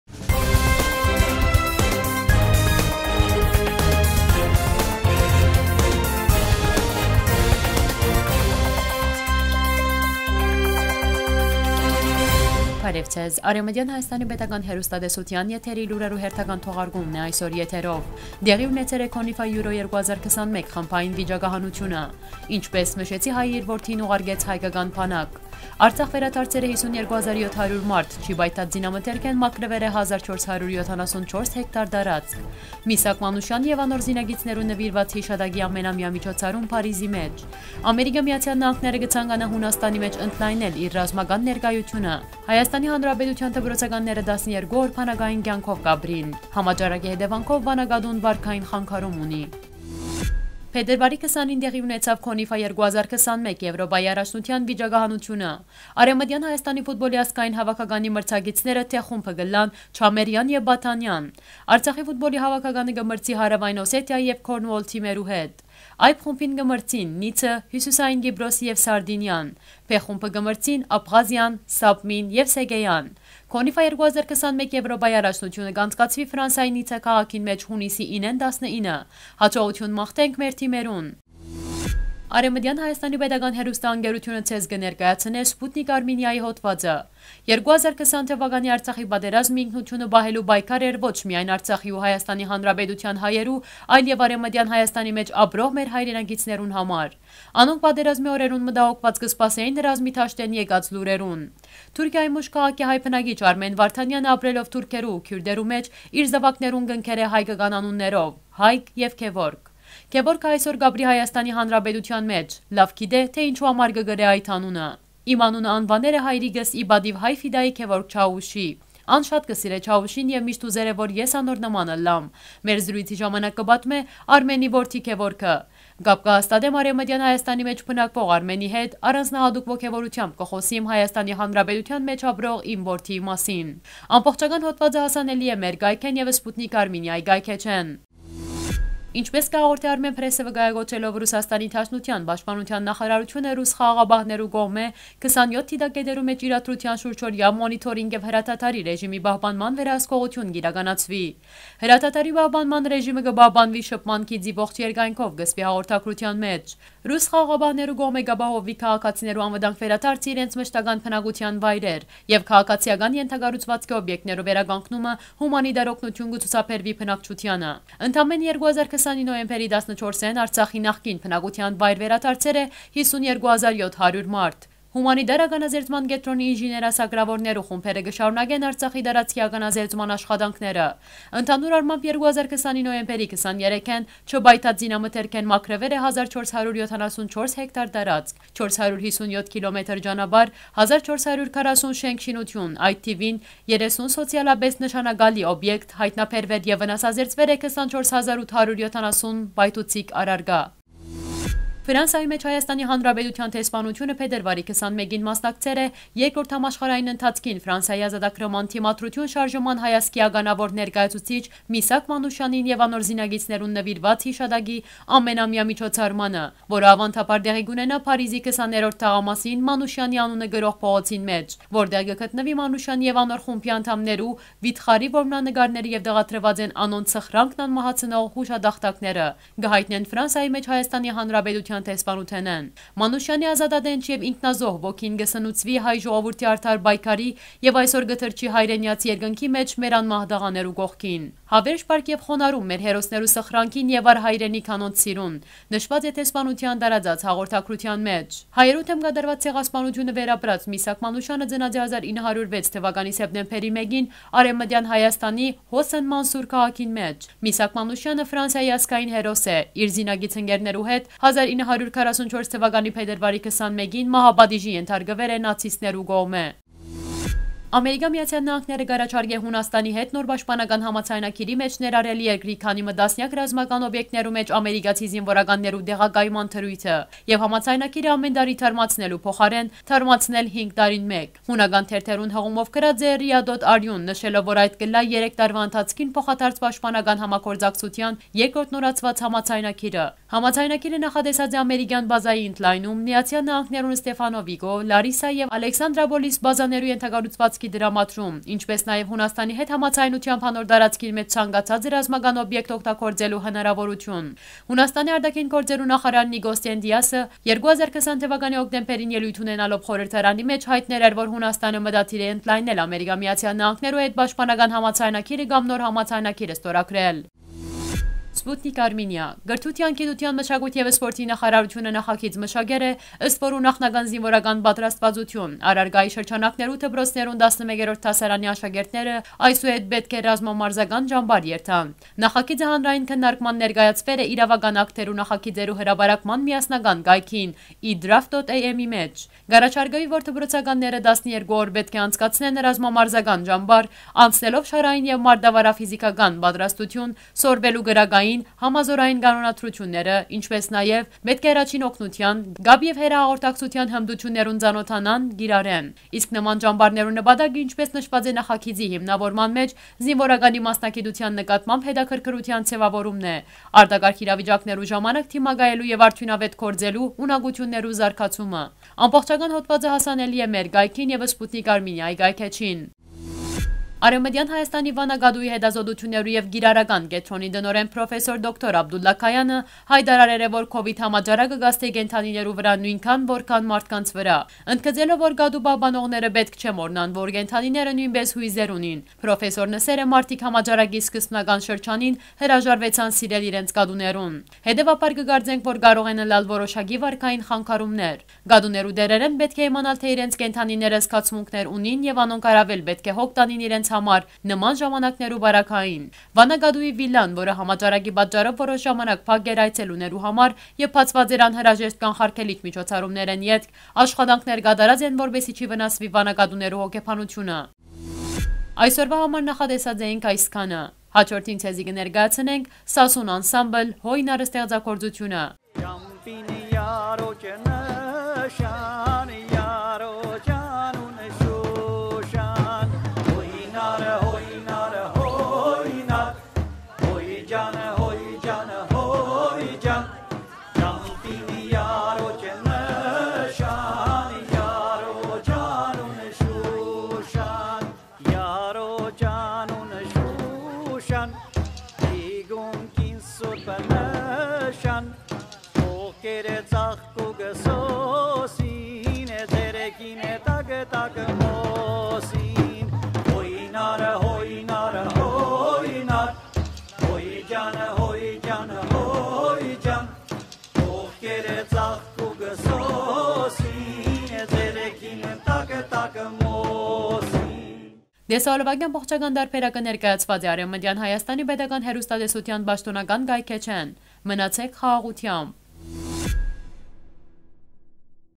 Արցախ վերադարձեր է 52 700 մարդ.Լուրեր 22 02 2021
1.Տեղի ունեցեր է CONIFA EURO 2021 Խմբային վիճակահանութիւնը
2. Ինչպէս մշեցի հայն իր որդիին ուղարկեց հայկական բանակ
3.Արցախ  վերադարձեր է 52 700 մարդ, չի պայթած զինամթերքէն մաքրուեր է 1 474 հա տարածք
4.Միսակ Մանուշեանին եւ անոր զինակիցներուն նուիրուած յիշատակի ամենամեայ միջոցառում՝ Փարիզի մէջ
5.ԱՄՆ-ն կը ցանկանայ Հունաստանի մէջ ընդլայնել իր ռազմական ներկայութիւնը․ ԶԼՄ-ներ
6.ՀՀ դպրոցականները 12 օր բանակային կեանքով կապրին
7.Համաճարակի հետեւանքով Վանա կատուն վարքային խանգարում ունի